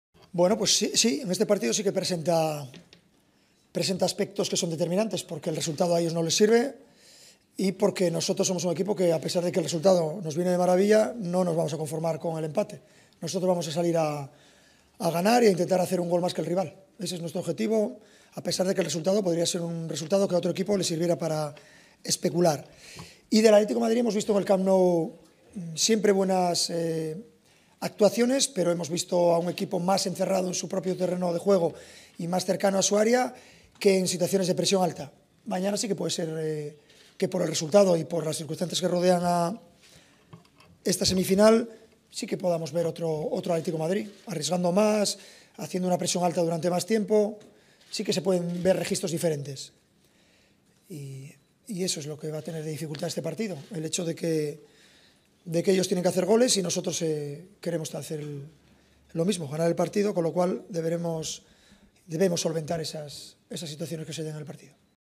Fc BARCELONE - Enrique «ON NE SE CONTENTERA PAS D'UN NUL»
Fc BARCELONA - Enrique "NOT TO BE MADE OF A NULL"

FOOT - Barcelona (1-2) on the lawn of Atlético Madrid in the Vicente Calderón Stadium in the semi-final of the King's Cup, will not try to manage his lead in the return leg. This is in any case what Luis Enrique, coach of the Blaugrana suggested.

FOOT - Barcelone qui s'est imposé (1-2) sur la pelouse de l'Atlético Madrid au Stade Vicente Calderón en demi-finale aller de la Coupe du Roi, ne cherchera pas à gérer son avance lors du match retour. C'est en tout cas ce qu'a laissé entendre Luis Enrique, l'entraîneur des Blaugrana.

LIKEZ & SUBSCRIBES...